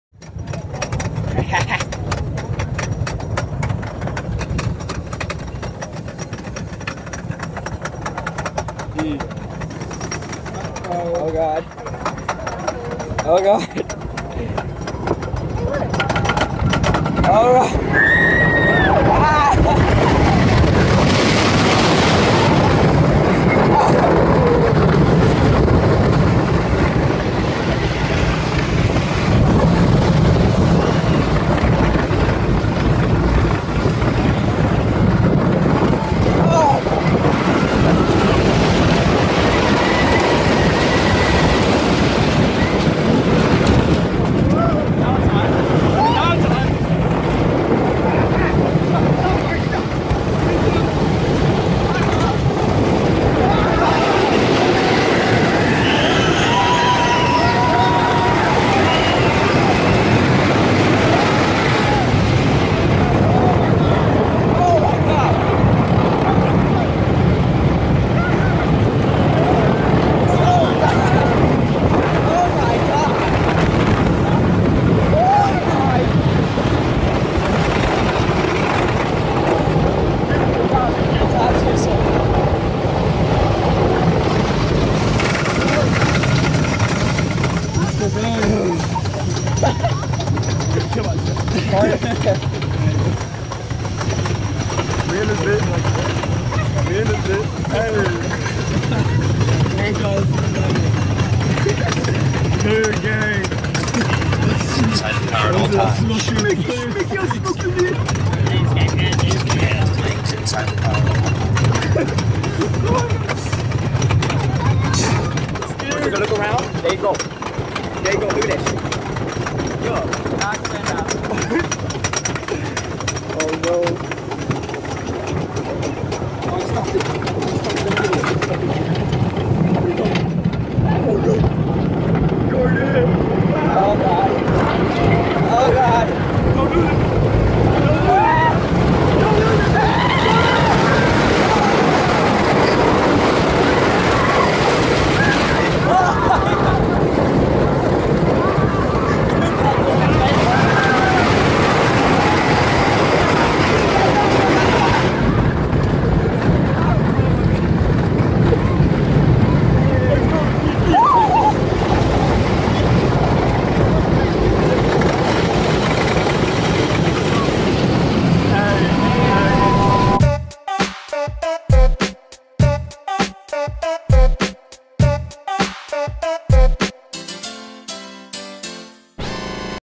Riding Kings Island's "The Beast" reactions from the past and the present
Summer of 2015 Kings Island VS Summer of 2018 Kings Island. I really do think I have gotten more autistic sense then, and its a real shame. Hope you enjoy our reactions!